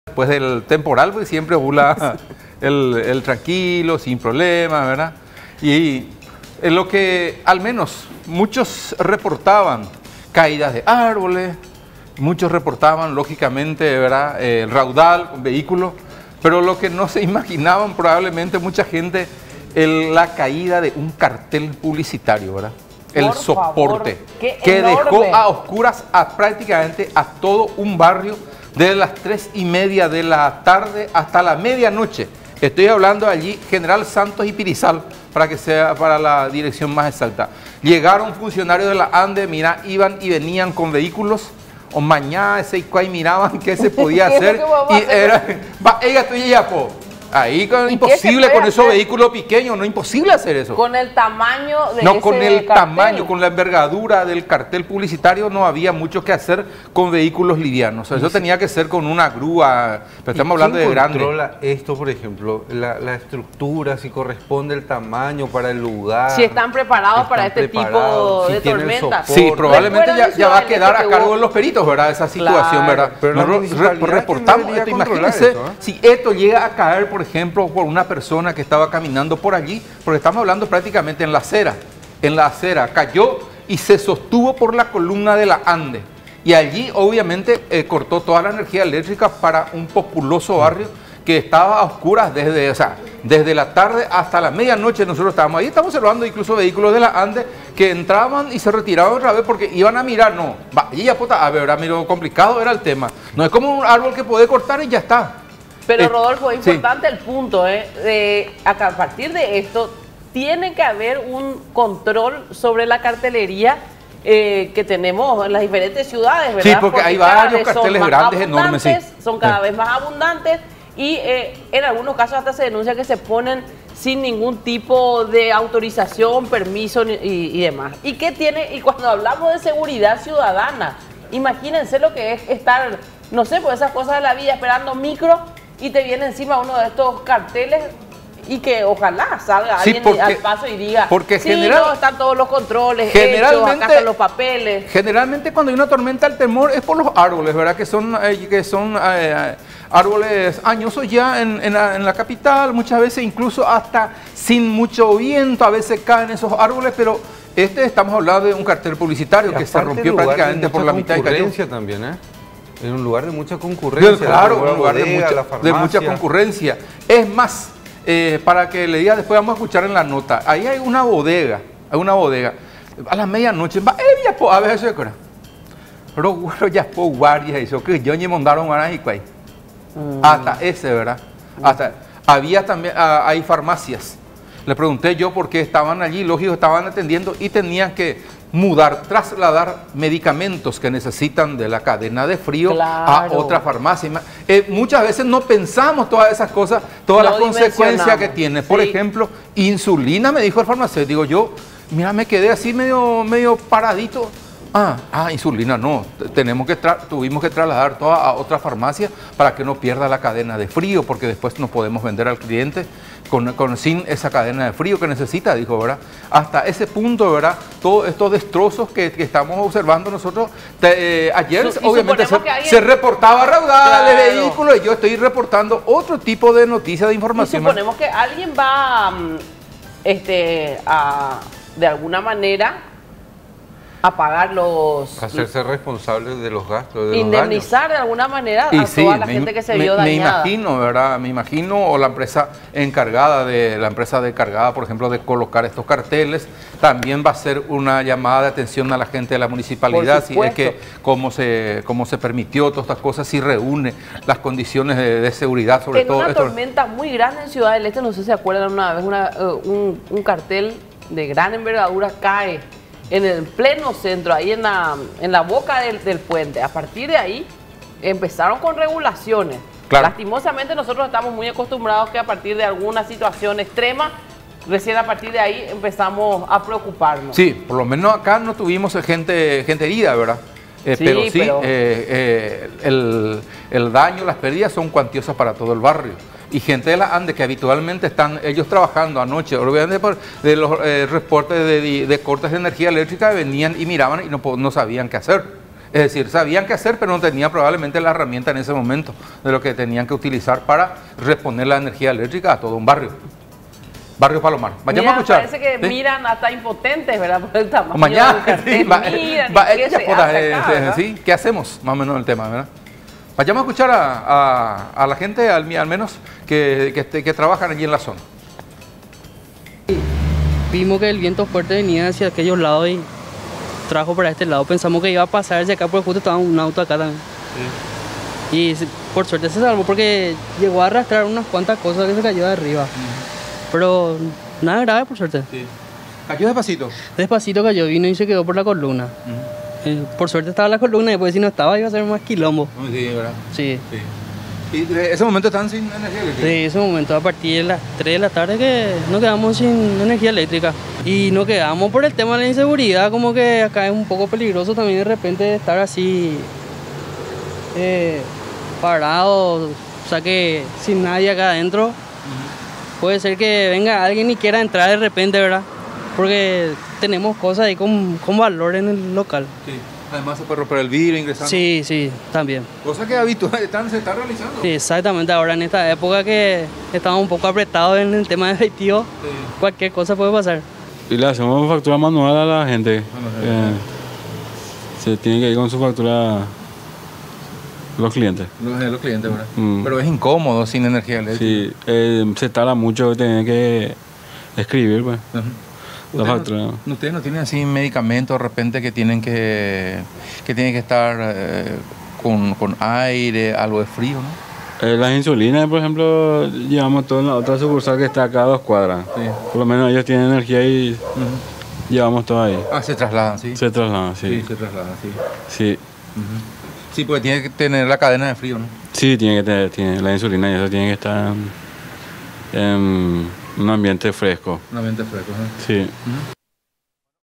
Caos por intenso temporal
El intenso temporal que se abatió sobre la capital y gran parte del país causó estragos a su paso, pues hubo cortes de energía, caída de árboles y hasta roturas de soportes publicitarios.